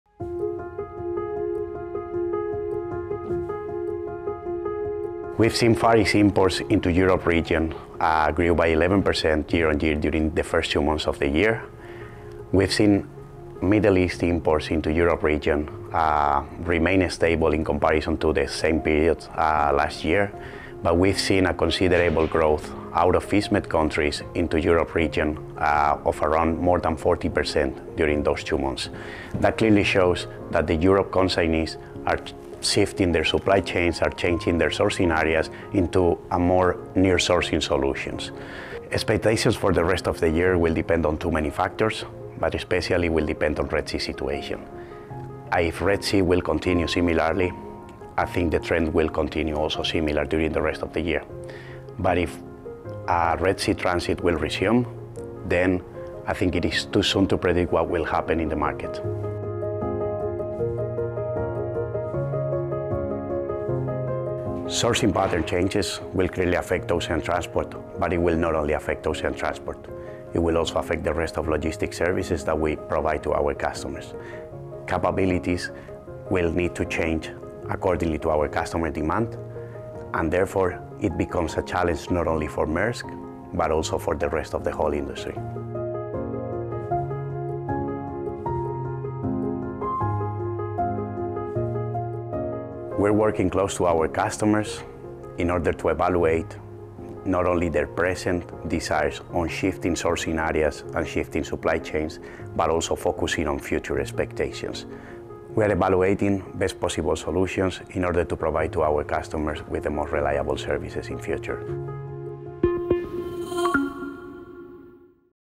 How Europe's Economy is Impacting Sourcing Strategies
Supply chain disruption is continuing to impact businesses across the world, so how are they going about adding new layers of resilience? Maersk Intra-Europe Market Manager, Vicente Ochando Roger, outlines the trends observed in the first part of 2024 and how sourcing strategies are evolving to combat logistics challenges.